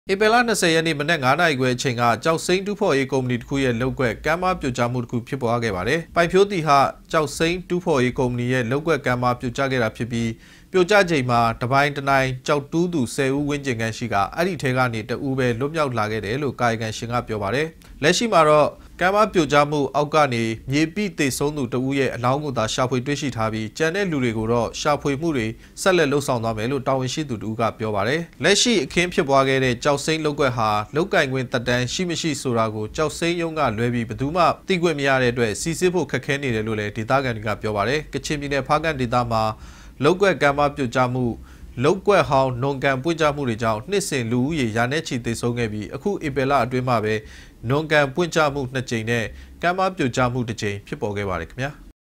DVB - ဖားကန႔္မွာ ေက်ာက္စိမ္း လုပ္ကြက္ ကမ္းပါး ၿပိဳက်လို႔ ေသဆုံးမႈေတြ ျဖစ္ေပၚ
ဧပြီလ ၂ဝ ရက်နေ့ မနက် ငါးနာရီခွဲအချိန်ခန့်က ကျောက်စိမ်းတူးဖော်ရေး ကုမ္မဏီတခုရဲ့ လုပ်ကွက် ကမ်းပါးပြိုကျမှုတခု ဖြစ်ပွားခဲ့ပါတယ်။ ပိုင်ဖြိုးသီဟ ကျောက်စိမ်းတူးဖော်ရေး ကုမ္မဏီရဲ့ လုပ်ကွက်ကမ်းပါး ပြိုကျခဲ့တာဖြစ်ပြီး ပြိုကျချိန်မှာ တပိုင်တနိုင် ကျောက်တူးသူ ဆယ်ဦးဝန်းကျင်ခန့်ရှိကာ အဲဒီထဲကနေ တစ်ဦးပဲလွတ်မြောက်လာခဲ့တာတယ်လို့ ကာယကံရှင်က ပြောပါတယ်။